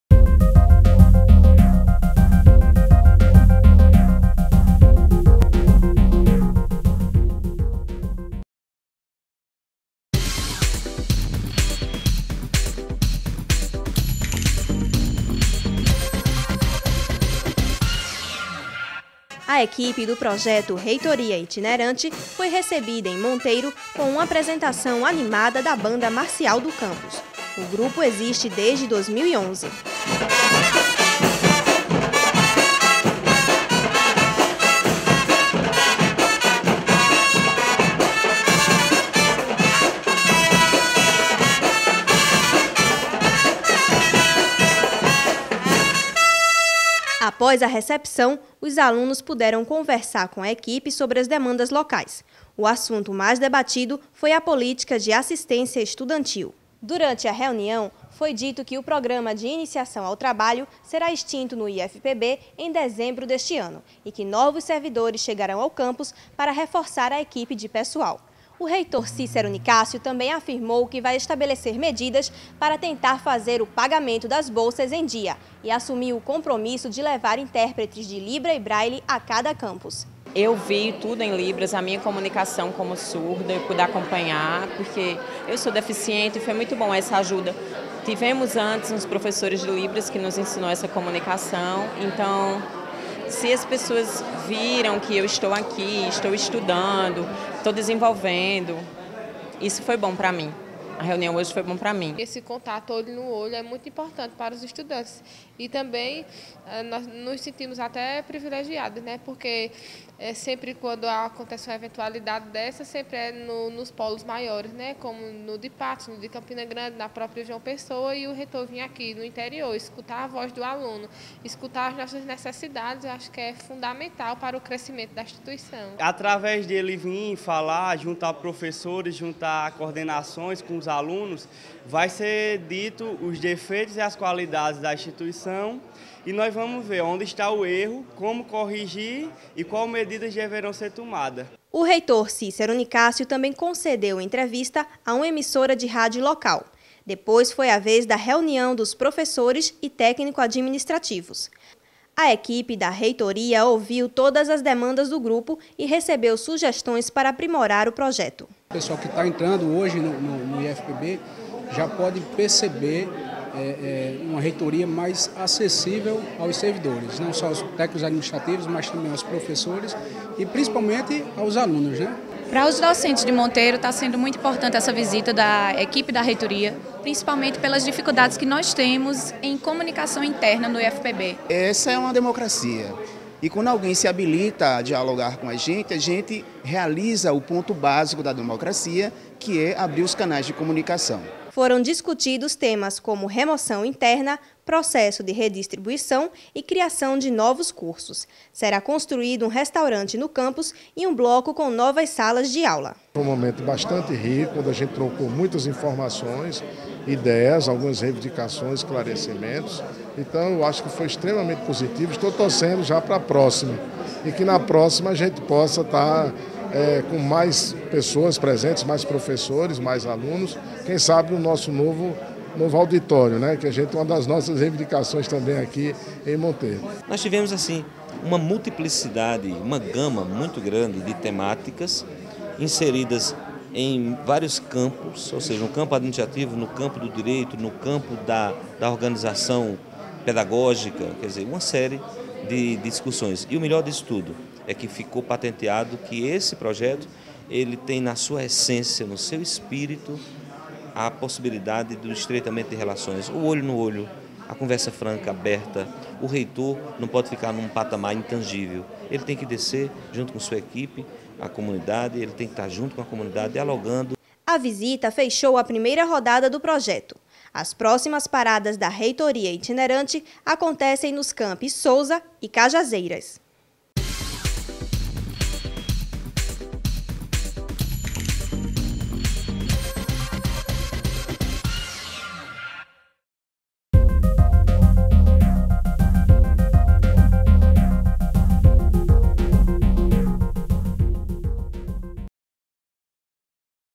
3ª Reitoria Itinerante - IFPB Campus Monteiro
3ª Reunião do Projeto da Reitoria Itinerante que ocorreu no IFPB Campus Monteiro, dia 17 de outubro de 2014.